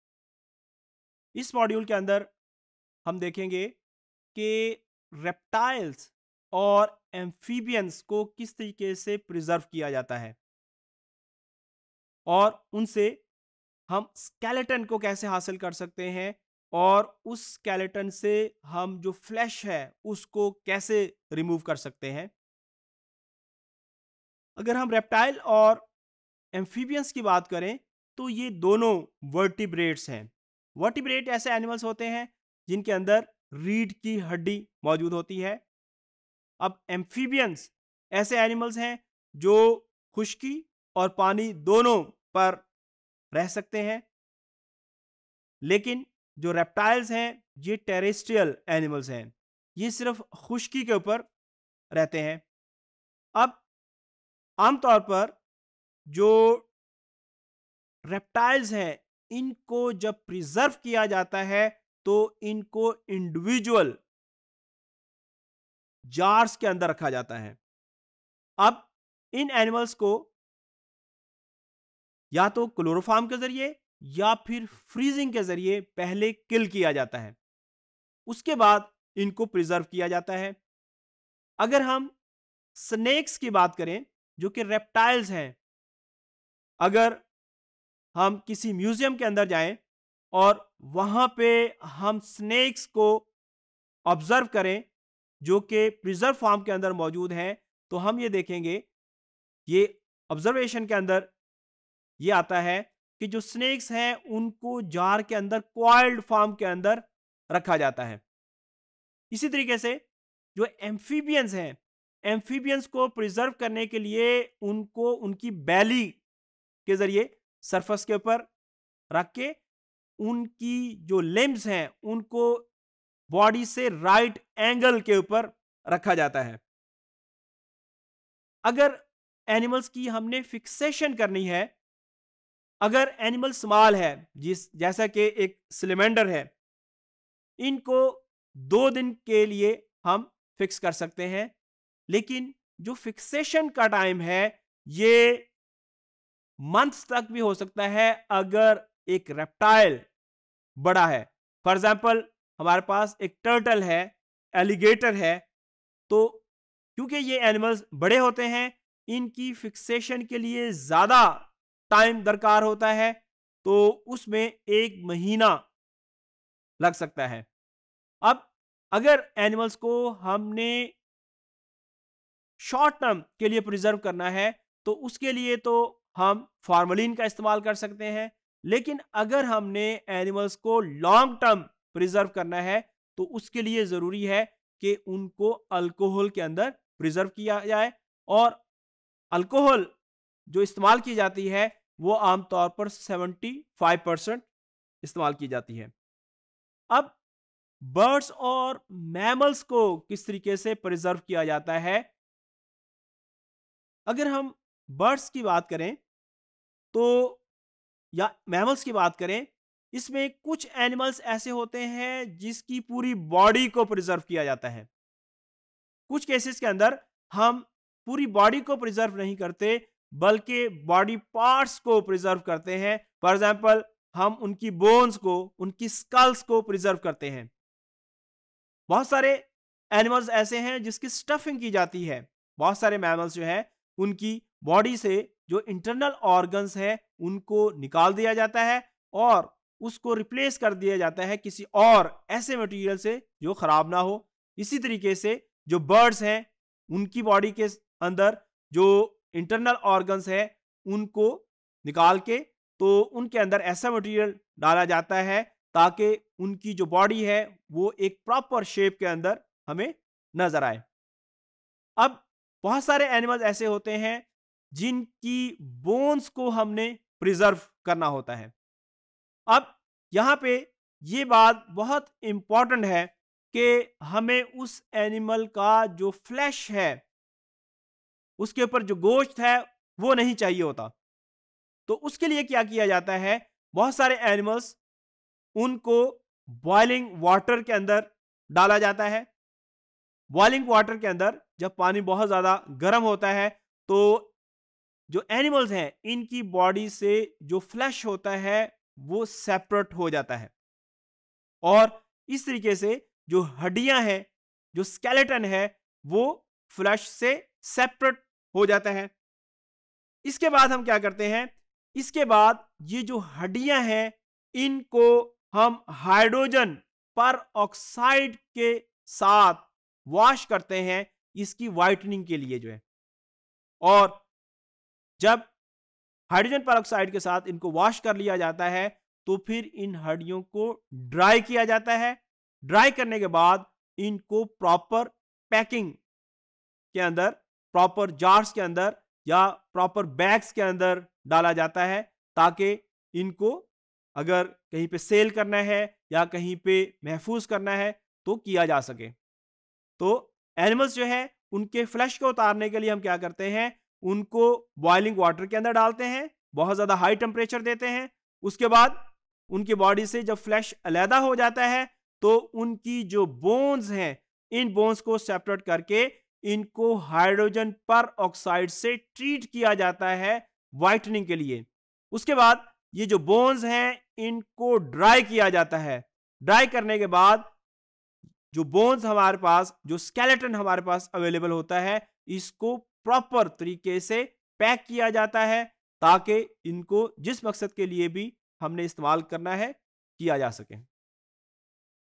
Methods of Animal Preservation 4 | Principles of Systematics | ZOO511T_Topic081
Topic081: Methods of Animal Preservation 4
by Dr. Muhammad Tahir